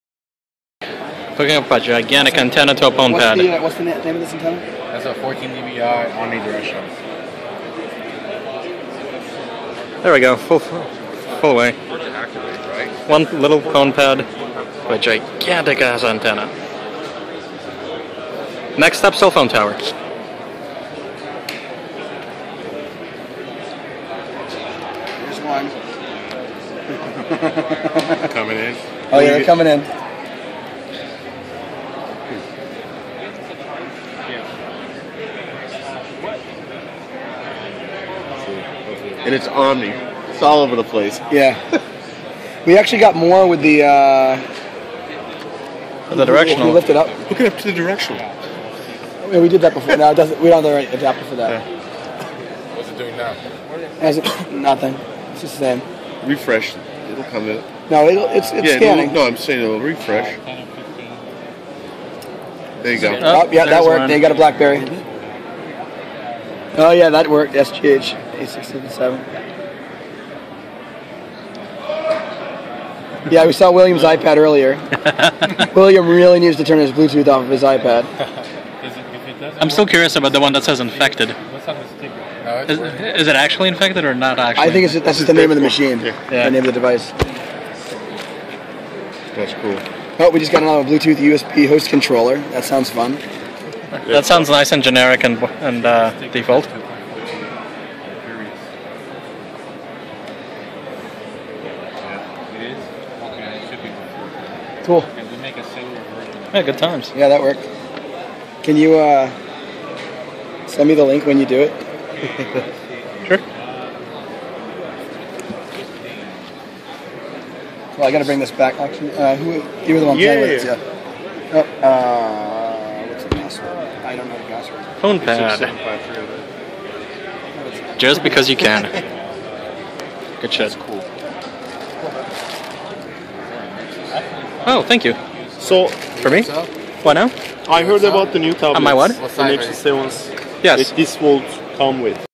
Pwn Pad, scanning bluetooth with GIANT antenna.
DEFCON 2013 Vegas, Pwnie Express